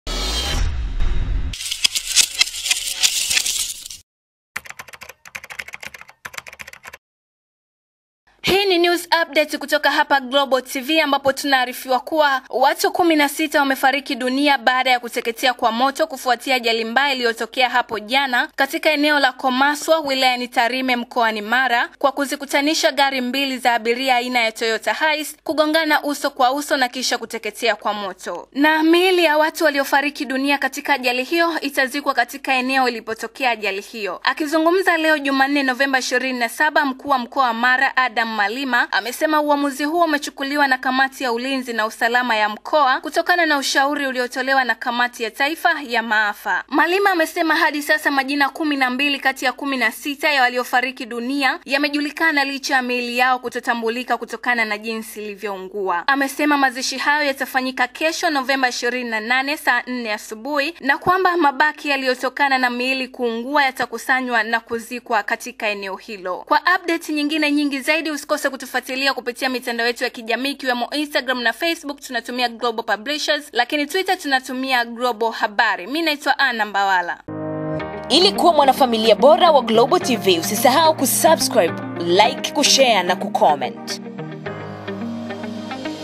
MSHTUKO! Moto ulivyoua watu 16, RC afanya maamuzi haya!
WATU 16 wamefariki dunia baada ya kuteketea kwa moto kufuatia ajali mbaya iliyotokea jana katika eneo la Komaswa maarufu kama Kwa-Gachuma lililopo wilayani Tarime mkoani Mara ambapo magari mawili ya abiria aina ya Toyota Hiace kugongana uso kwa uso na kuteketea kwa moto.

Miili ya watu waliofariki dunia katika ajali hiyo itazikwa katika eneo ilipotokea ajali hiyo .

Akizungumza leo Jumanne Novemba 27, 2018 Mkuu wa Mkoa wa Mara, Adam Malima,  amesema uamuzi huo umechukuliwa na kamati ya ulinzi na usalama ya mkoa huo kutokana na ushauri uliotolewa na Kamati ya Taifa ya Maafa.